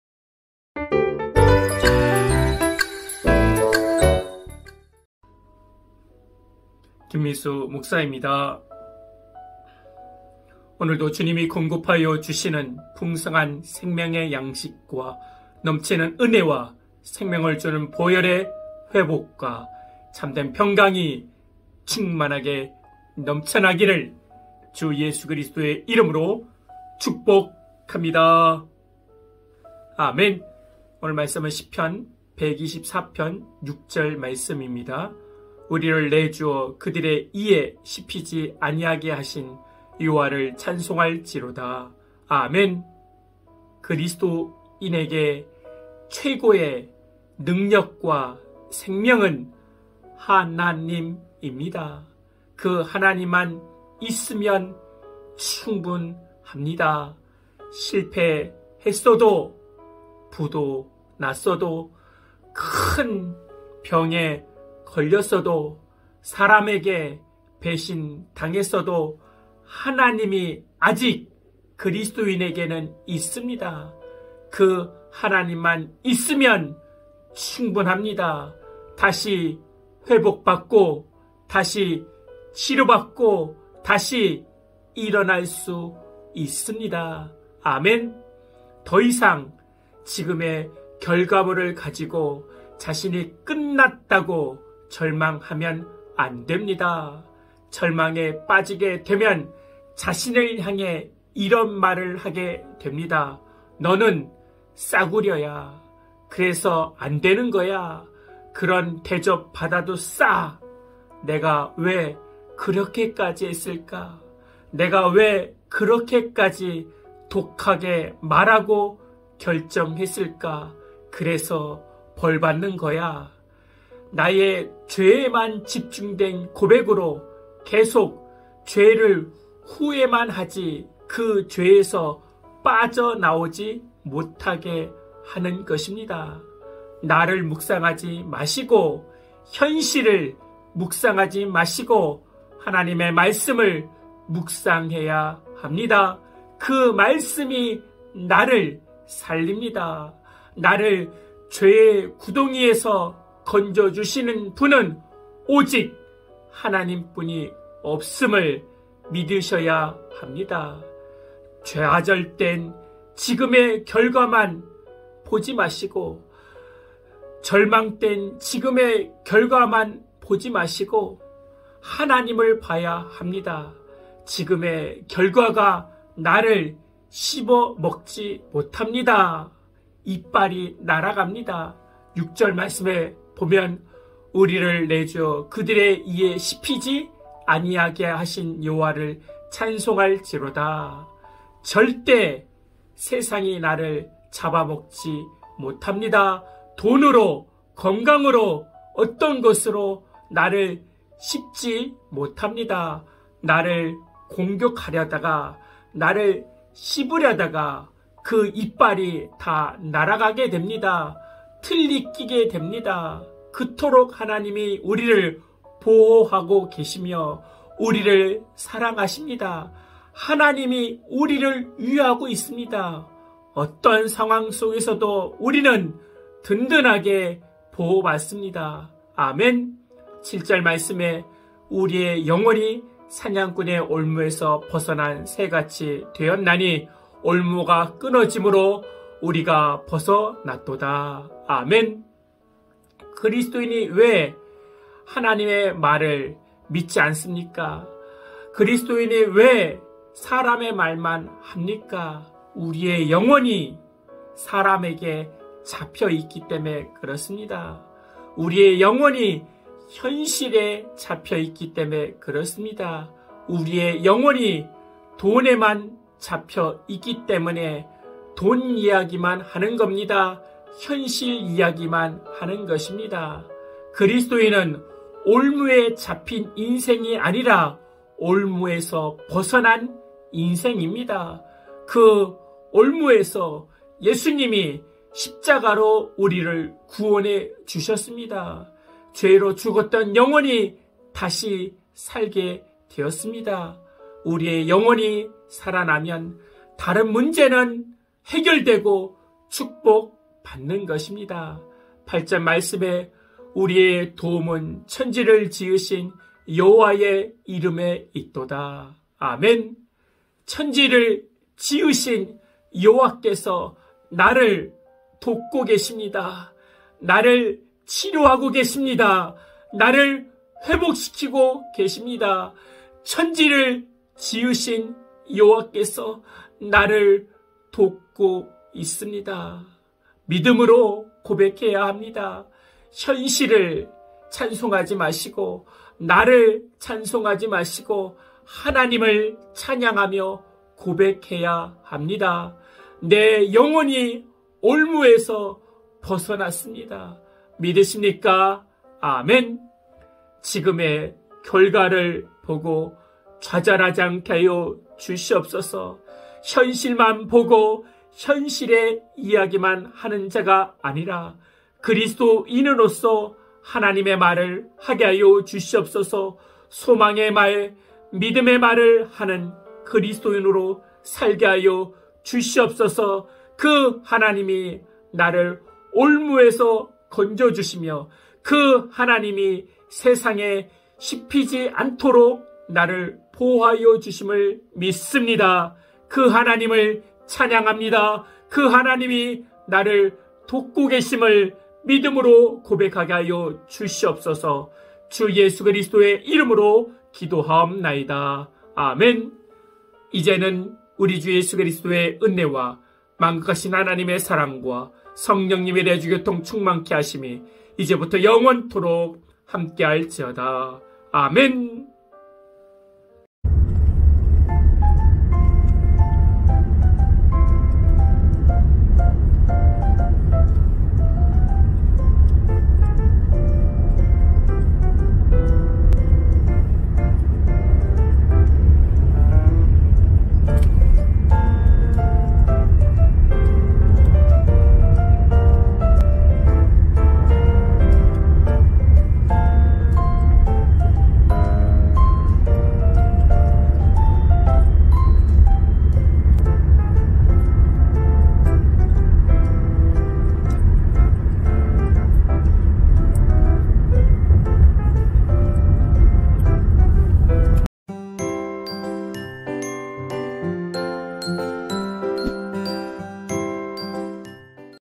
나를_씹는_다고 시124편강해(2)
나를 씹는 다고?
시124:6우리를 내주어 /그들의 이에 씹히지 아니하게 하신/ 여호와를 찬송할지로다

그리스도인의 최고에 능력과 생명은 하나님입니다. 그 하나님만 있으면 됩니다. 실패했어도, 부도가 났어도, 큰 병에 걸렸어도, 사람에게 배신당했어도 하나님만 있으면 충분합니다. 다시 일어날 수 있습니다.

더 이상 지금의 결과물을 가지고 자신을 끝났다고 절망하면 안 됩니다. 절망에 빠지게 되면  자신을 향해 이런 말을 하게 됩니다. 너는 싸구려야! 그래서 안 되는 거야!, 그렇게 대접받아도 싸!, 내가 왜 그렇게 까지 했을까?, 내가 왜 그렇게 까지 독하게 말했을까?, 그래서 벌 받는 거야! 나의 죄에만 집중된 고백으로, 계속 죄를 후회만 하지 그 죄에서는 빠져나오지 못하게 하는 것입니다.

나를 묵상하지 말고, 현실을 묵상하지 말고 하나님의 말씀을 묵상해야 합니다.
그 말씀이 나를 살립니다. 나를 구덩이에서 건져주는 것입니다.

좌절된 지금의 결과가 나를 잡아먹지 못합니다. 나를 씹어서 먹는다고 하다가 이빨이 날아갑니다.
6우리를 내주어 그들의 이에 씹히지 아니하게 하신 여호와를 찬송할지로다

절대 세상이 나를 잡아먹지 못합니다. 나를 씹지 못합니다. 나를 공격하려다 이빨이 다 나가 틀리하게 됩니다. 하나님이 우리를 사랑하십니다. 하나님이 우리를 위하고 있습니다. 어떤 상황에서도 구원받습니다.

7우리의 영혼이/ 사냥꾼의 올무에서 벗어난 새 같이 되었나니/ 올무가 끊어지므로 우리가 벗어났도다

그리스도인이 왜 하나님의 말을 하지 않고 사람말만 하는 것입니까?
우리의 영혼이 사람에게 잡혀서 사람말만 하는 것입니다. 우리의 영혼이 현실에게 잡혀서 현실이야기만 하는 것입니다. 우리의 영혼이 돈에 잡혀서 돈이야기만 하는 것입니다.
그리스도인은 울무에 잡힌 인생이 아니라 울무에서 벗어난 인생입니다. 그 울무가 예수님으로 말미암아 끊어졌습니다. 죄로 죽었던 영혼이 다시 살아났다는 것입니다. 영혼이 살아나면 다른 문제는 해결되고 축복받는 것입니다.

8우리의 도움은 천지를 지으신 여호와의 이름에 있도다.

천지를 지으신 여호와께서 나를 돕고 있습니다. 천지를 지으신 여호와께서 나를 돕고 있습니다.

믿음의 고백을 해야 합니다. 현실을 찬송하지 말고 하나님을 찬양하며 고백하야 합니다. 내 영혼이 올무에서 벗어났습니다. 믿으십니까? 아멘

기도
내가 가진 결과가 끝이 아니고 그리스도인에게 하나님이 있음을 고백하게 하옵소서 더 이상 나를 묵상하지 말고 나의 현실을 묵상하지 말고 하나님의 말씀을 묵상하여 힘을 얻게 하시고 승리하게 하시고 치료받아 일어나게 하옵소서 주 예수 그리스도의 이름으로 기도합니다. 아멘